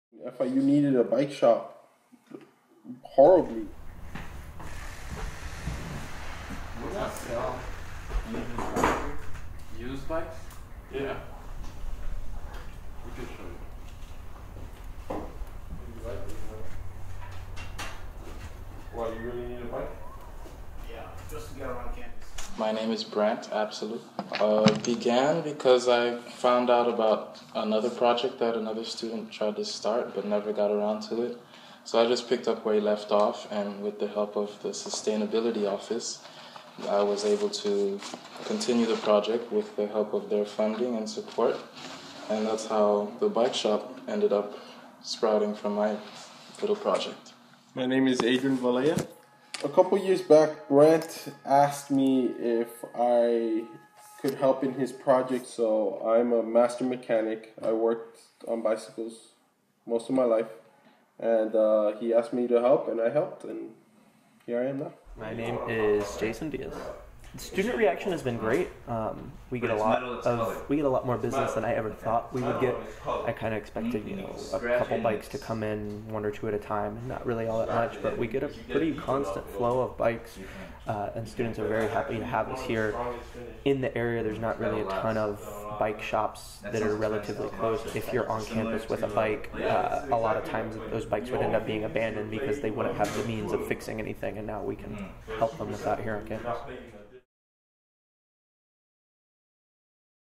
Soundslide: FIU Bike Shop
Audio and photos for senior multimedia project class, FIU, Spring 2013 •
The FIU Bike Shop has been on campus for the past two or three semesters